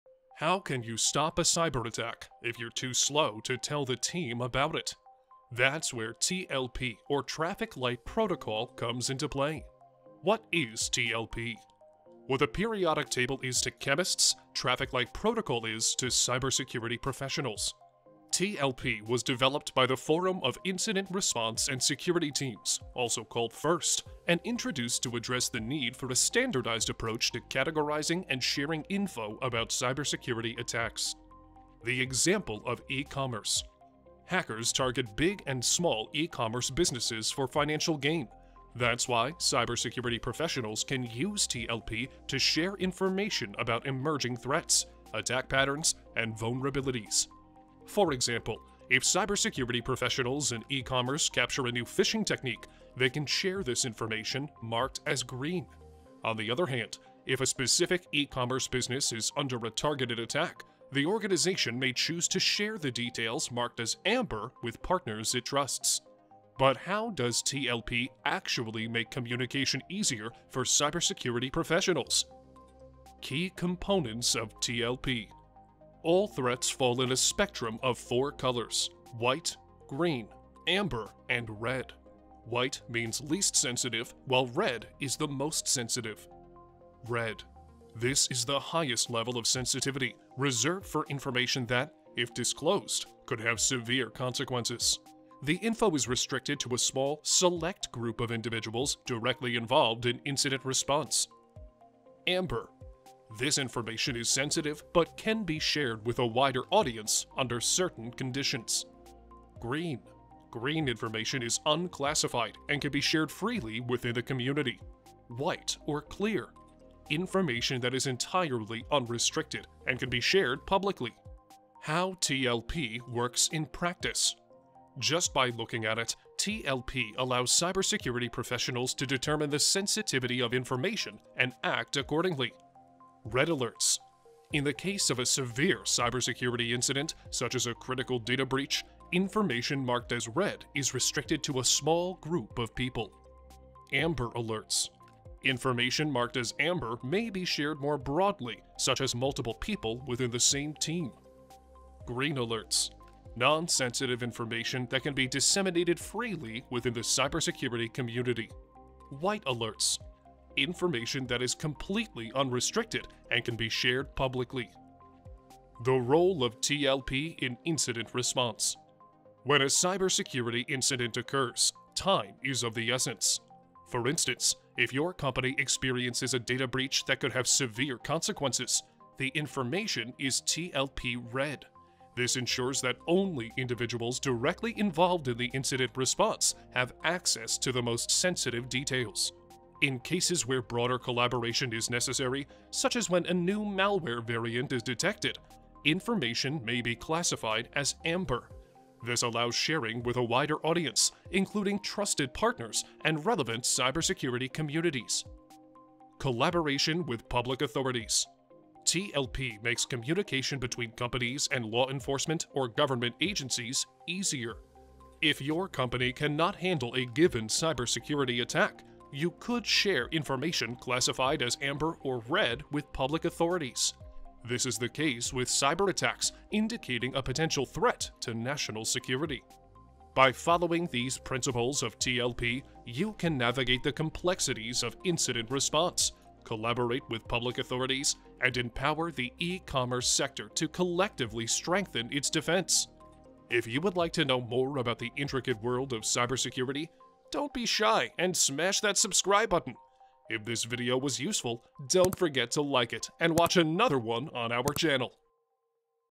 Ready, Steady, Go: Traffic Lights Protocol (TLP) in Cybersecurity
Effective communication and information sharing play pivotal roles in thwarting potential threats. One of the mechanisms that facilitate this exchange of information while ensuring responsible and secure dissemination is the Traffic Light Protocol (TLP). The Traffic Light Protocol was conceived as a framework to standardize the sharing of sensitive information within the cybersecurity community

In this video, we delve into the intricacies of TLP, exploring its origins, principles, and how it aids in the classification and distribution of sensitive information within the cybersecurity realm.